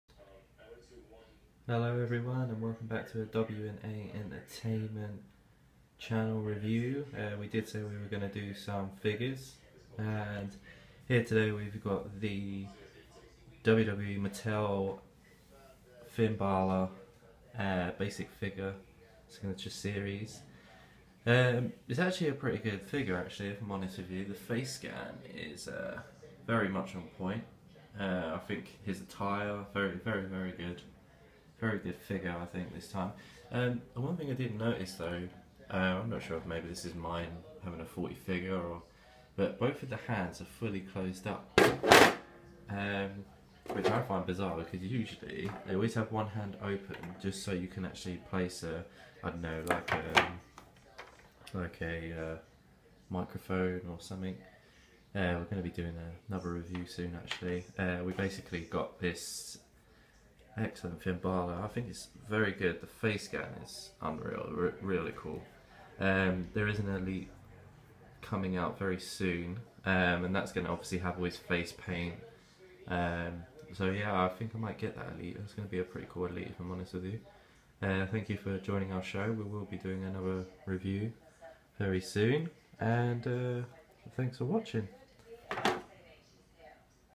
WWE Finn Balor figure review
Today we reviewed the WWE Basic figure Finn Balor.

Happy viewing!!